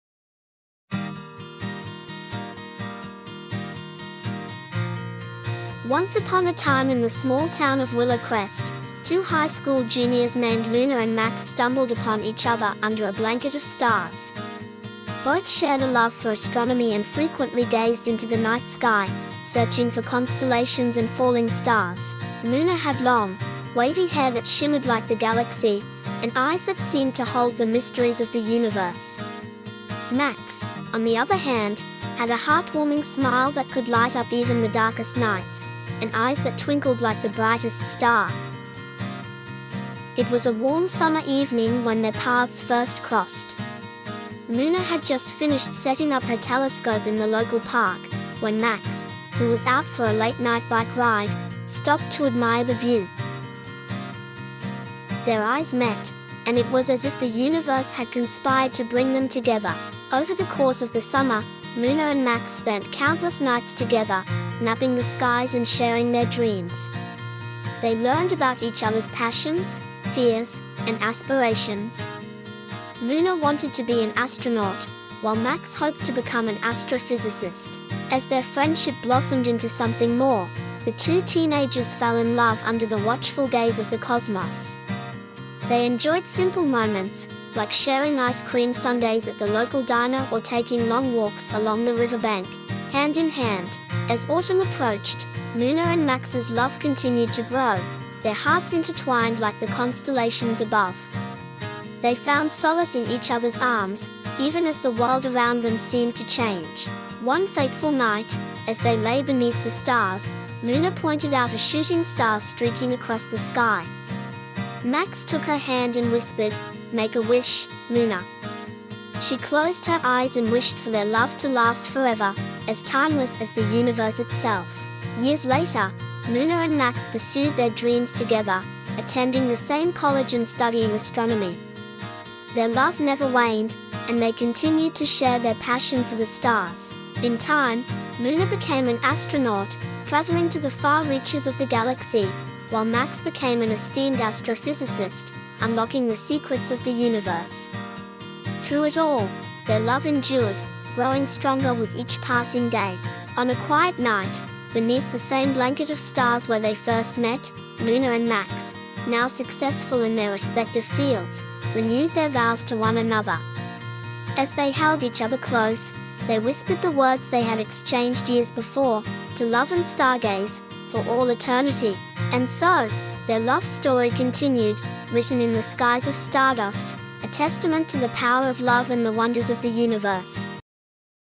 Skies of Stardust
Join Luna and Max in their cosmic love story, where they share a passion for the stars and a love that transcends time and space. Follow their journey as they pursue their dreams of exploring the universe, hand in hand, with Max's signature glasses adding a touch of intellectual charm. Witness their unwavering love amidst the wonders of the cosmos, as they gaze at shooting stars, map the skies, and make heartfelt wishes. Their enduring bond serves as a beautiful reminder of the power of love and the infinite beauty of the universe. LoveAmongTheStars StargazingLoveStory CosmicRomance AstronomyLoveStory GalacticLoveStory CelestialRomance AstroLoveStory"